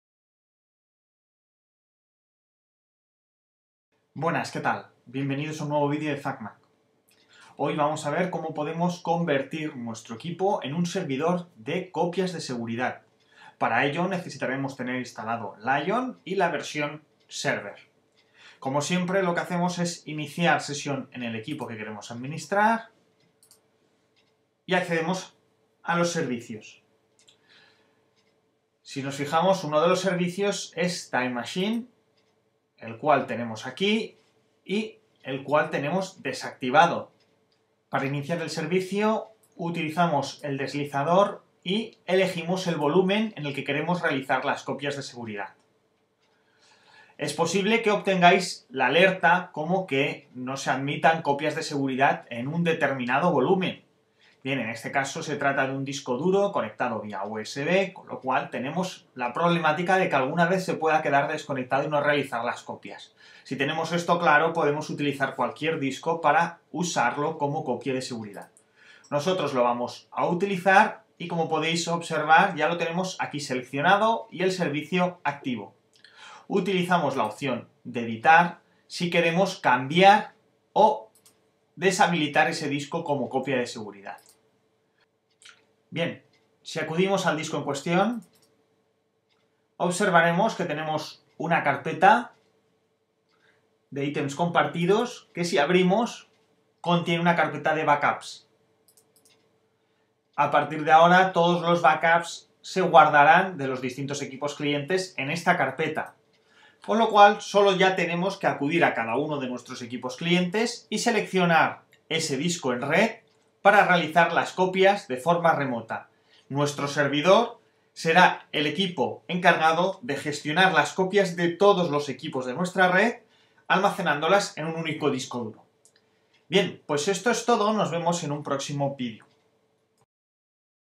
Todo sobre MacOS X Lion Server - Servidor de copias de seguridad
Con MacOS X Lion Server será muy sencillo para el usuario configurar su equipo para centralizar todas las copias de seguridad de los equipos clientes. Gracias a este servicio, convertimos el disco duro seleccionado en el servidor, en un almacén de copias que funcionará de la misma manera que lo hace TimeCapsule.